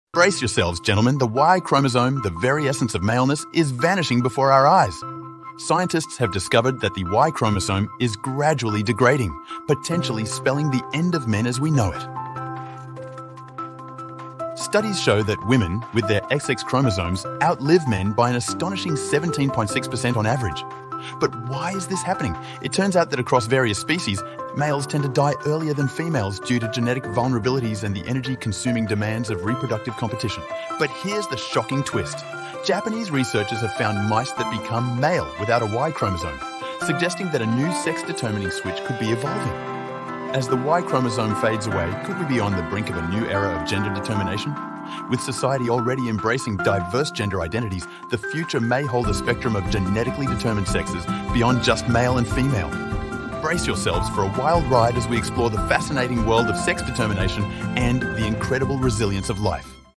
Is This the End of Men? 😱👨 Disappearing Y Chromosome
The Y chromosome is disappearing, potentially marking the end of men as we know it. But as one sex-determining system fades, a new one may be evolving, promising a future with a spectrum of genetically determined genders.